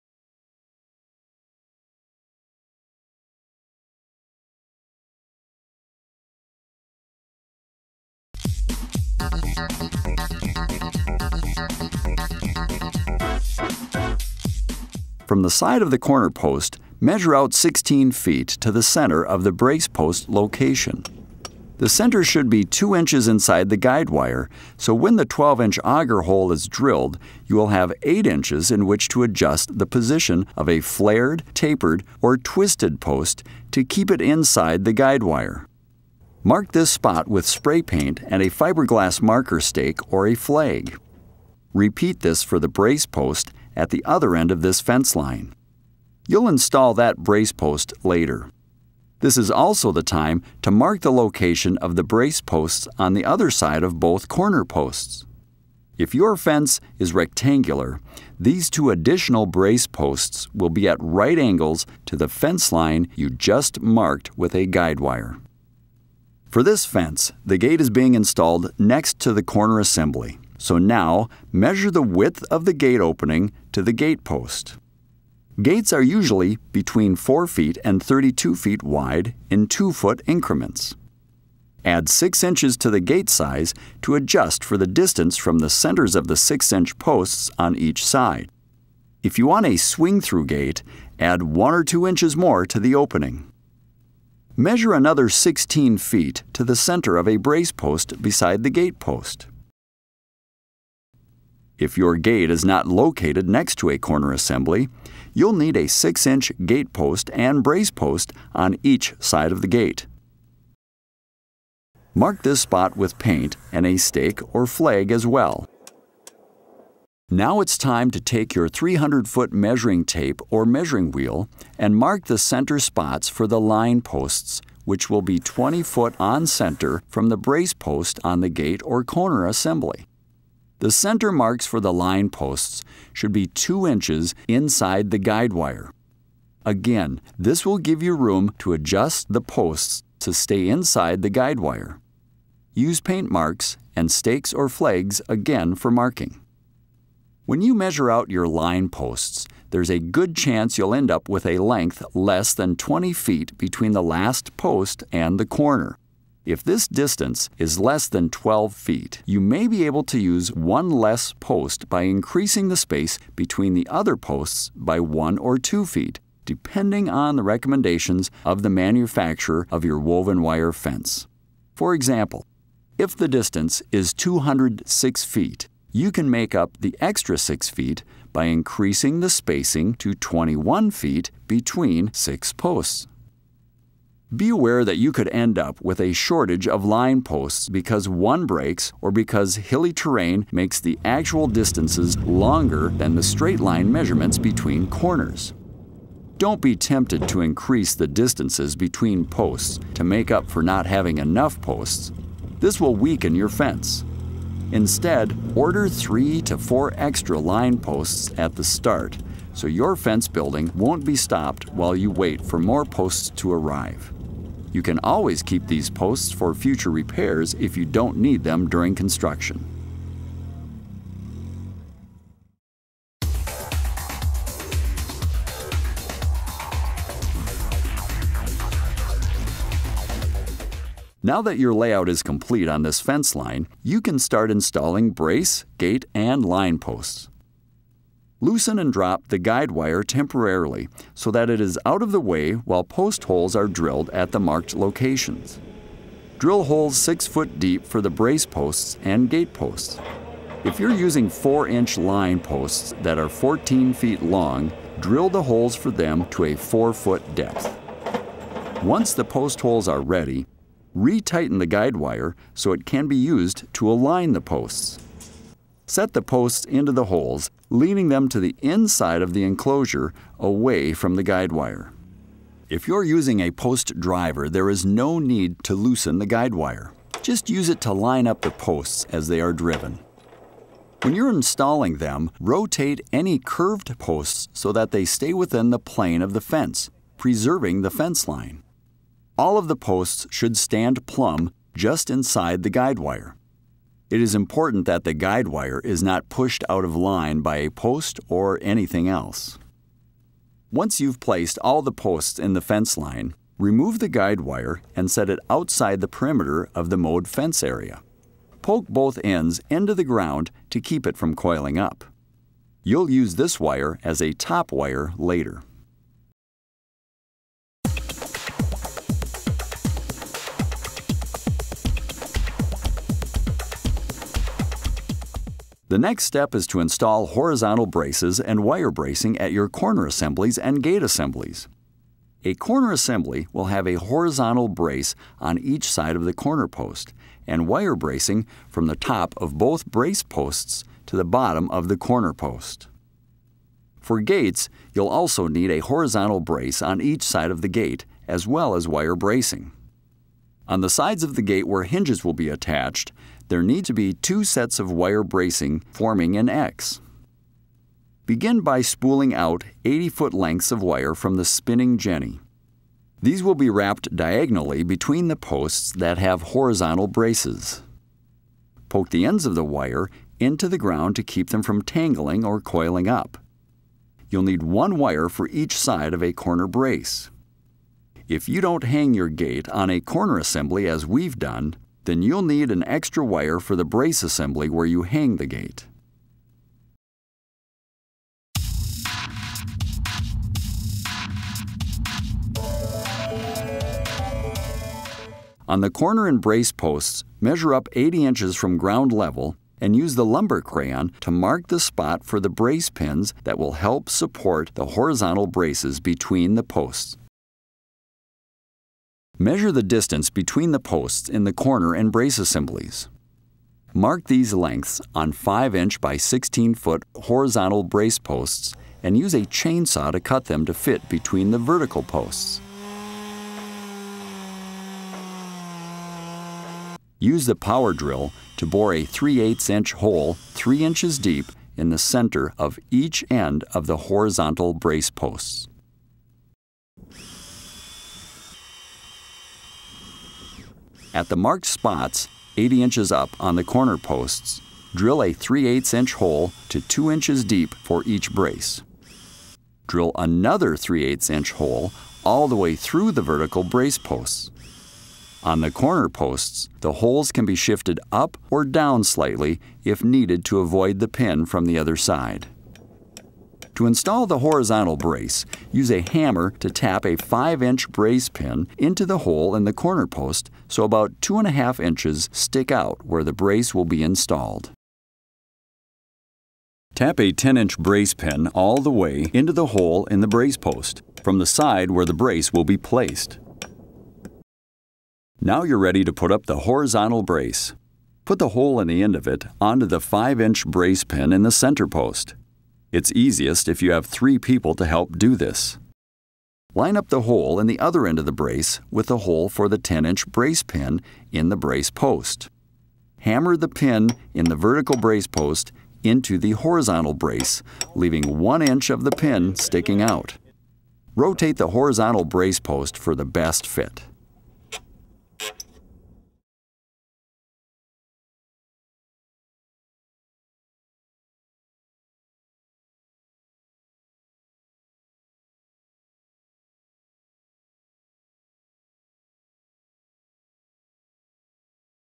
Wildlife Fence Construction – Part 3
This training video demonstrates how to install a wildlife fence to prevent deer from feeding on agricultural crops.
Part 3
- Measuring Post Locations
- Step 2: Installing Posts
- Step 3: Installing Bracing at Corners and Gates
- Horizontal Brace